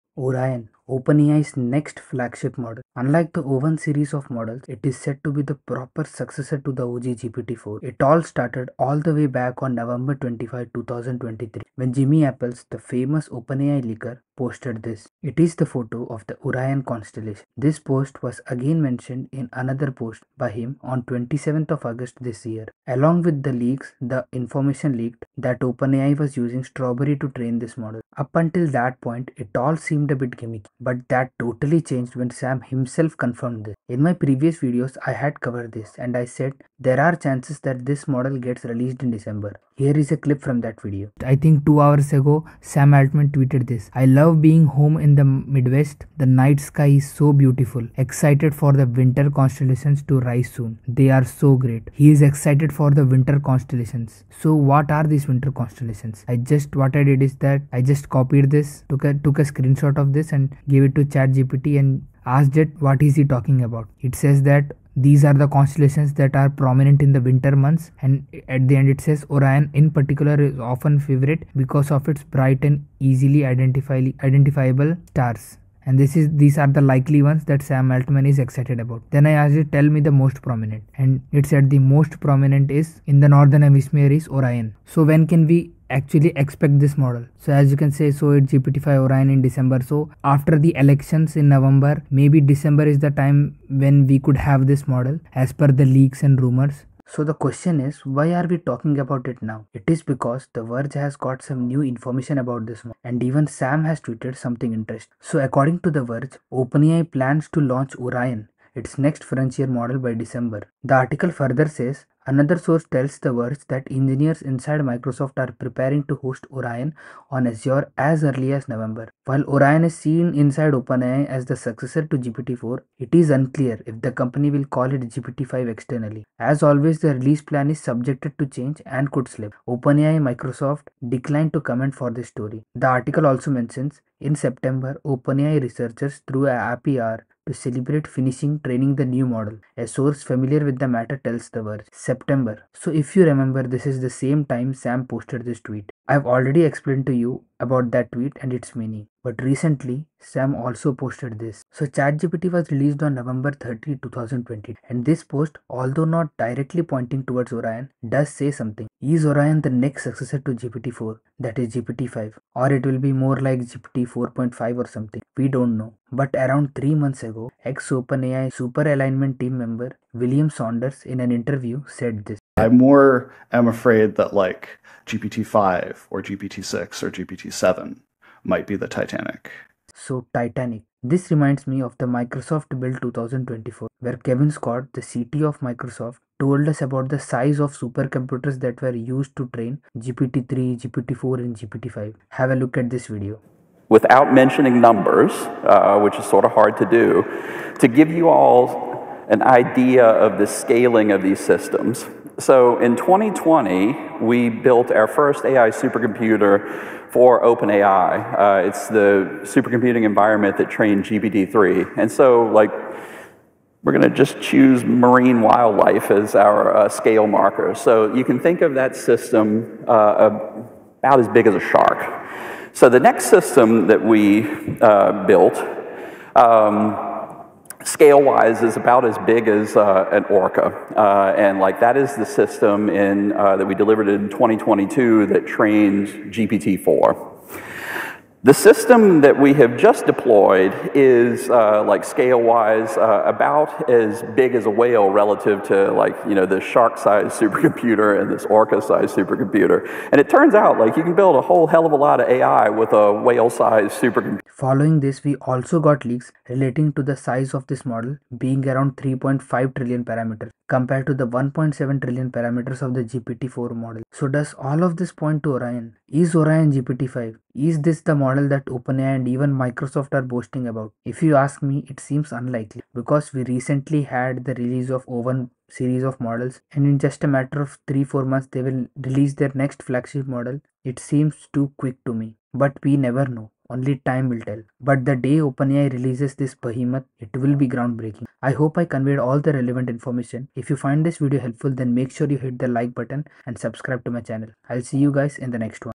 🚨BREAKING News! OpenAI's plans to launch ORION (GPT 5) !
In this video, we dive into the exciting revelations surrounding Orion, OpenAI's next flagship AI model, seen as the potential successor to GPT-4. We explore the initial leaks, Sam Altman's intriguing tweets, and the implications of scaling AI models like never before. Could we be on the brink of a groundbreaking release this December?

Your support helps us keep delivering the latest news and analysis.